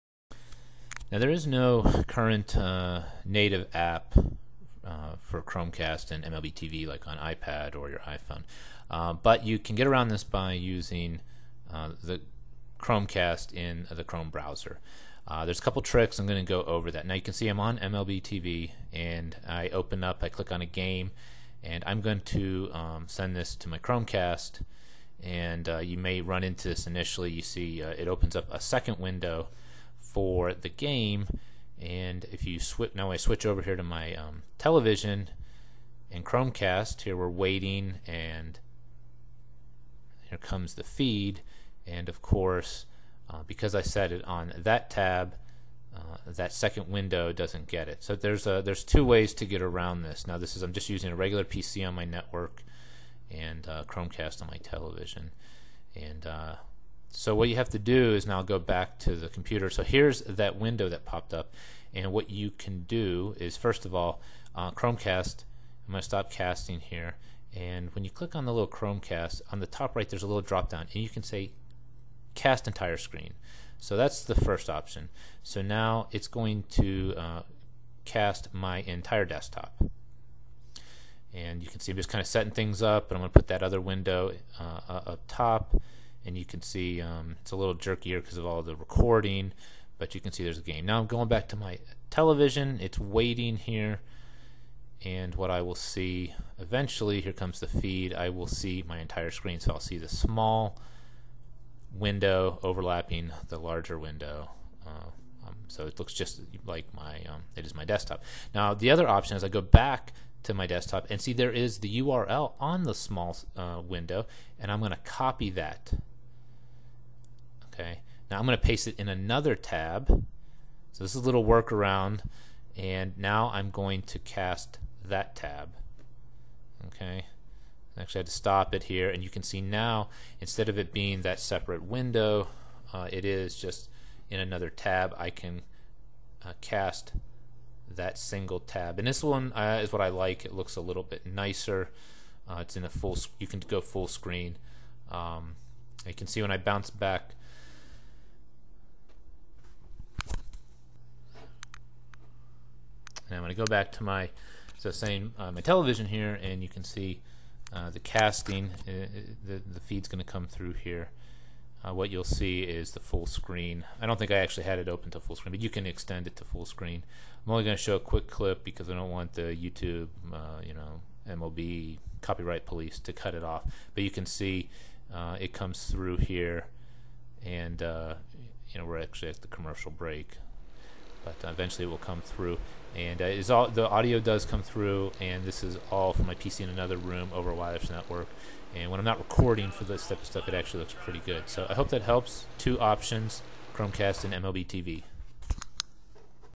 How to cast MLB.TV with Chromecast
UPDATE:  As of 4/17/14 MLB app officially support Chromecast!

Two options for Chromecasting MLB.TV to your TV.  No native MLB app uses chromecast yet, so you need to run it in a browser and either cast the entire desktop or the specific tab in Google Chrome.